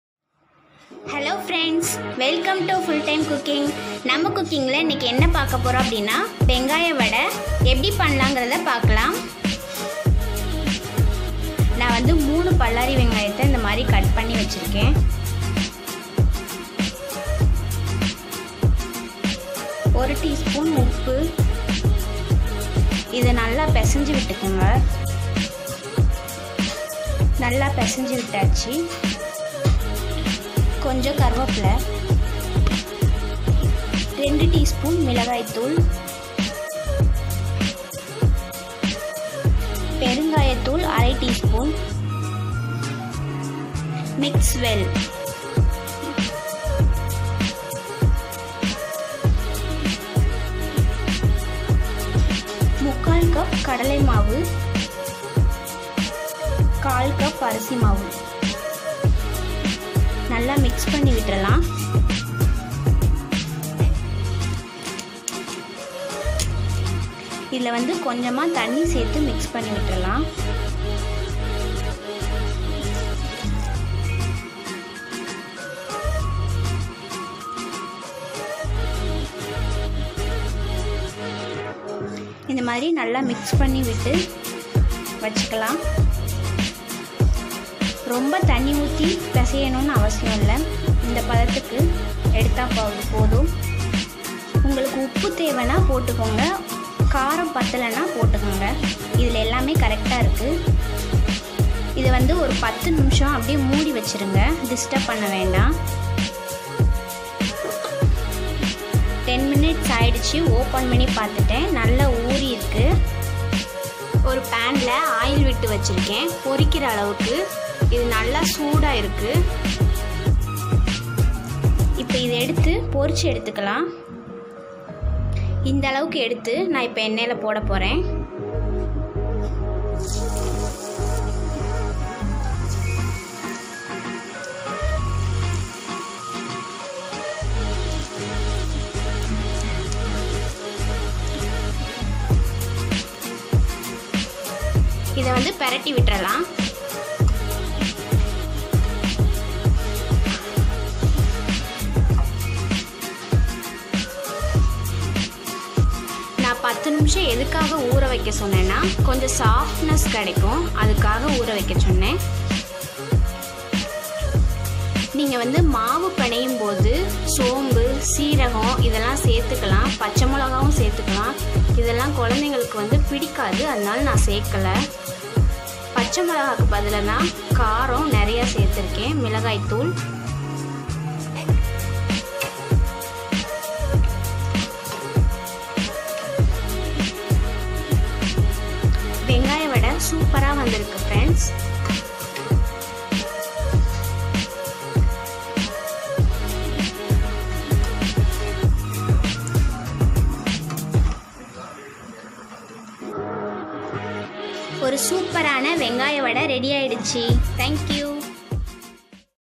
மொறு மொறு வெங்காய வடை | Tea Kadai Vadai | Evening Snacks Recipe | Vengaya Vadai Recipe in Tamil
Ingredients:

Rice flour - 2sp
Red chilli powder - 1 1/2 tsp
Asafotida - 1/4 tsp
Cumin powder - 1/2 tsp
Salt - as for taste
Oil